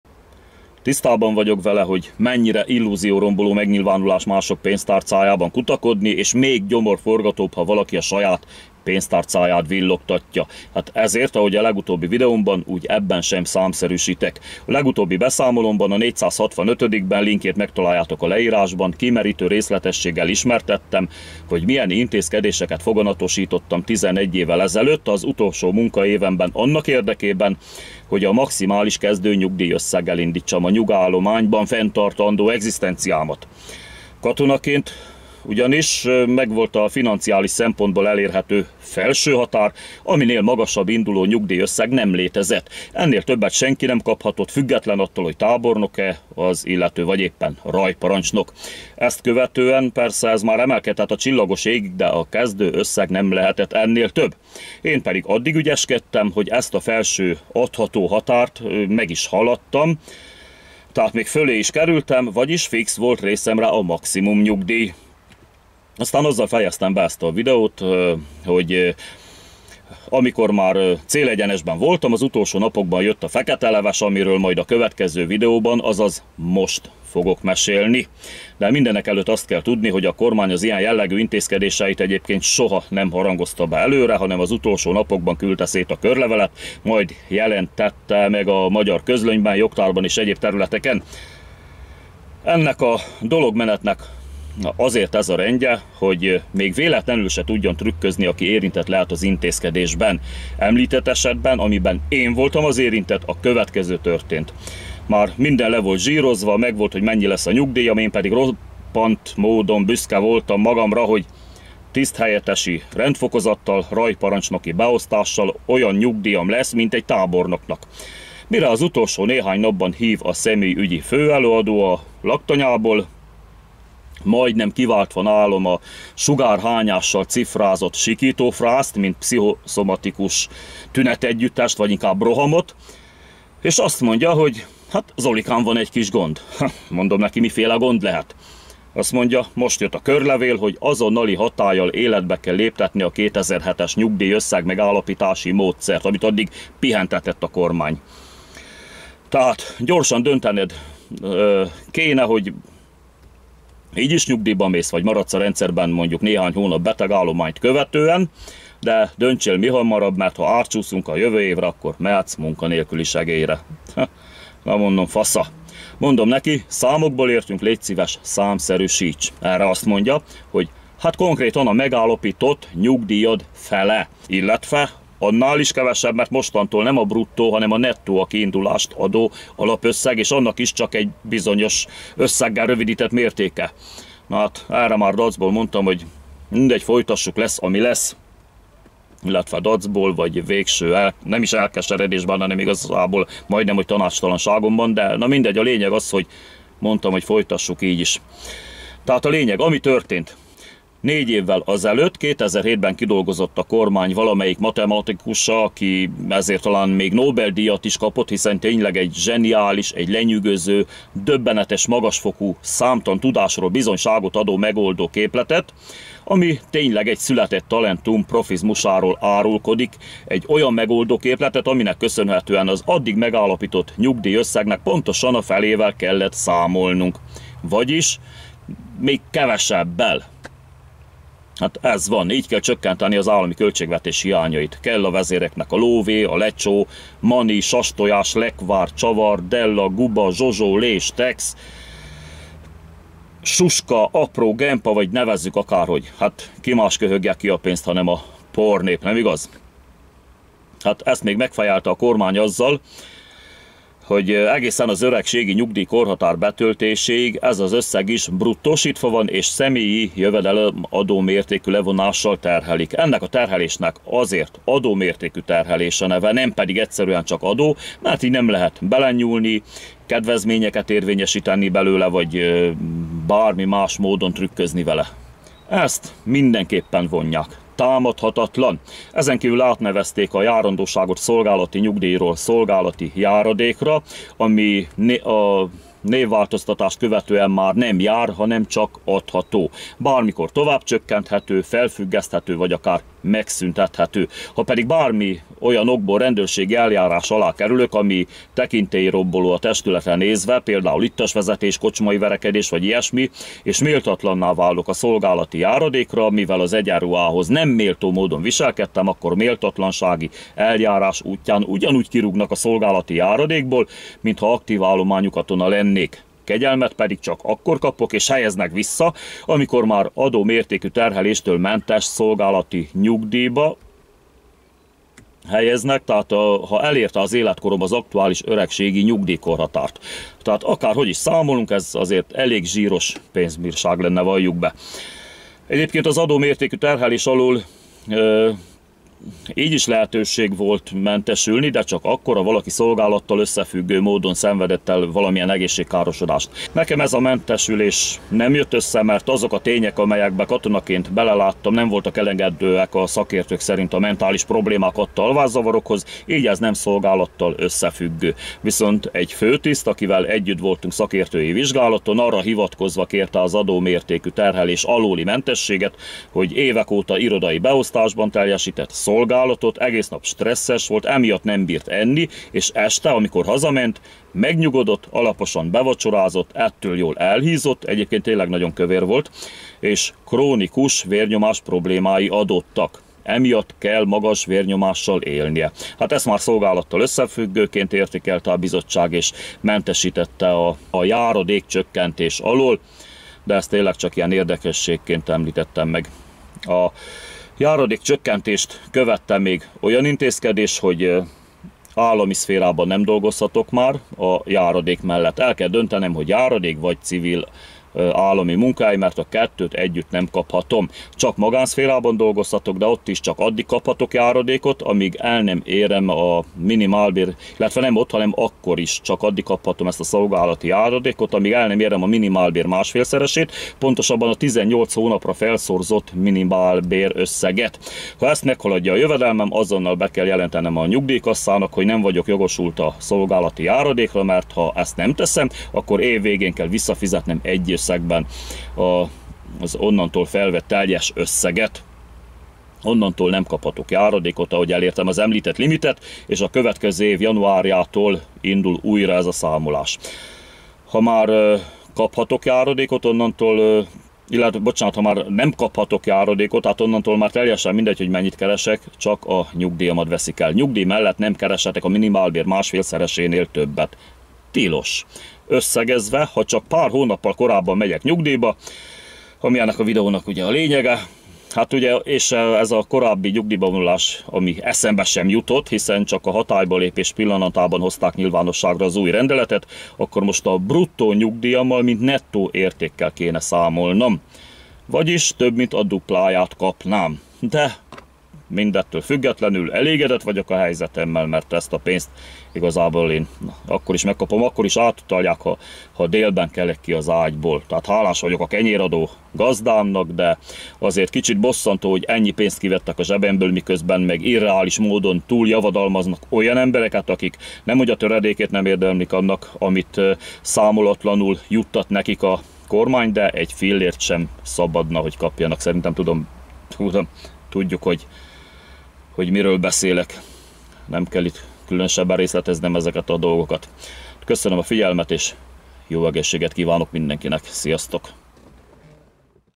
466. Így vette el a kormány a katonai nyugdíjam felét
Hivatásos katona voltam. Jól indult a nyugdíjazásom, aztán egy kormányrendelet megkurtította az egyenlet végén szereplő eredményt.